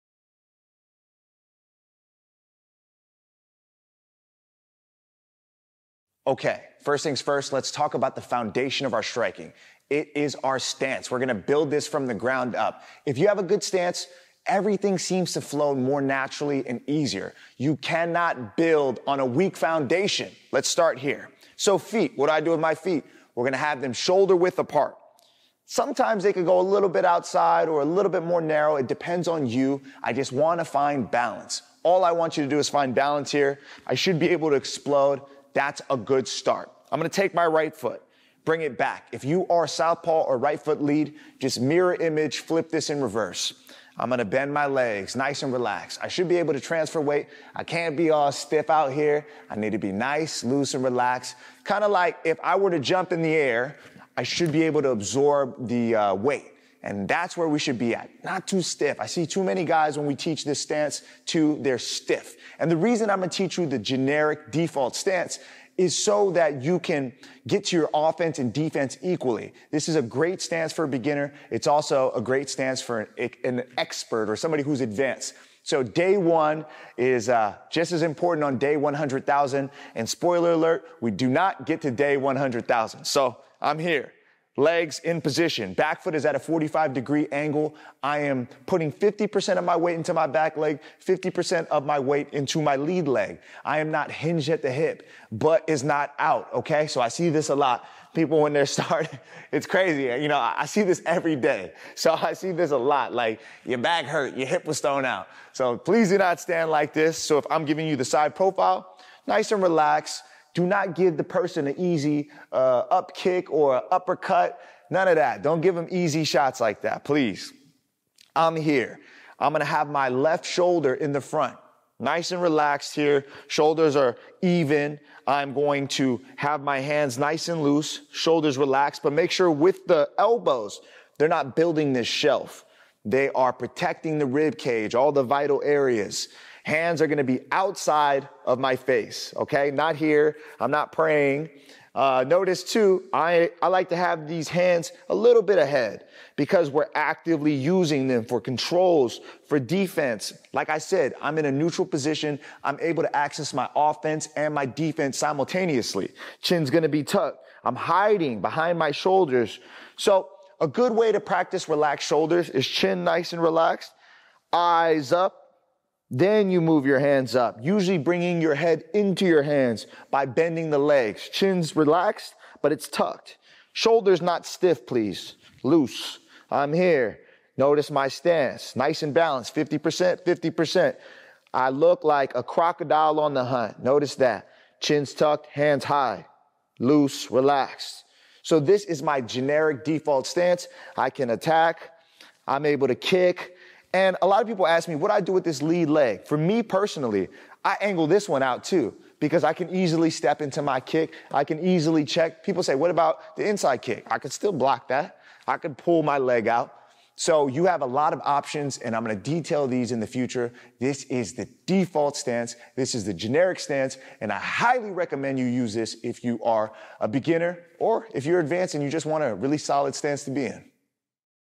Step-by-Step: Establishing Your Muay Thai Stance
First, let's talk about the foundation of our striking: the stance. Building this from the ground up is crucial. A good stance allows everything to flow more naturally and easily. You can't build on a weak foundation.

Let's start with the feet. Position them shoulder-width apart, though you can adjust slightly wider or narrower depending on your comfort. The key is to find balance. You should be able to move explosively from this stance.

For right-footed fighters, or those using a Southpaw stance, mirror these instructions. Bend your legs comfortably, staying relaxed and able to transfer weight. Avoid stiffness. Think of how you would absorb weight when jumping.

The stance I teach is a generic, default one, suitable for beginners and experts alike. It's equally good for offense and defense.

Here's how to position yourself: Place your back foot at a 45-degree angle, distributing your weight evenly between your front and back legs. Avoid hinging at the hip or sticking it out, which I see often in beginners. This incorrect posture can cause back and hip problems.

Stand with your side profile to your opponent, keeping your left shoulder in front, shoulders even, and hands relaxed but ready. Your elbows should protect your ribcage, and your hands should be outside of your face, not too close. Keep them slightly ahead for active defense and control.

Maintain a neutral position, enabling access to both offense and defense. Tuck your chin and hide behind your shoulders. Practice this with relaxed shoulders and chin, keeping your eyes up. Your stance should be balanced, with a 50-50 weight distribution, resembling a crocodile on the hunt: alert, chin tucked, hands high, and relaxed.

Regarding the lead leg, I personally angle it out for easier stepping into kicks and checking. This stance offers various options for defense and attack, and I'll detail more in the future.

This default stance is highly recommended for beginners and advanced fighters seeking a solid, reliable stance.